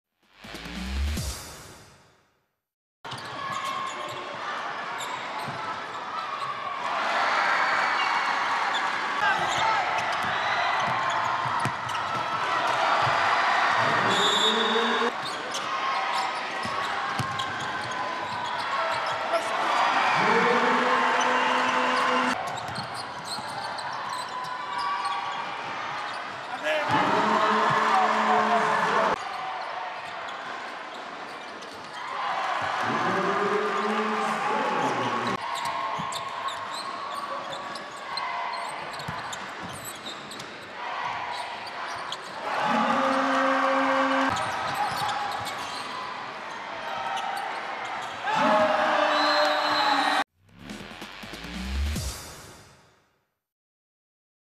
James Bell show! (Cedevita - Petrol Olimpija, 7.12.2018)
James Bell of Cedevita simply shined in the game against Petrol Olimpija, especially in the game’s ending when it mattered the most. He scored 23 points, grabbed 7 rebounds, dished out 2 assists, stole 1 ball and had an excellent valuation of 31.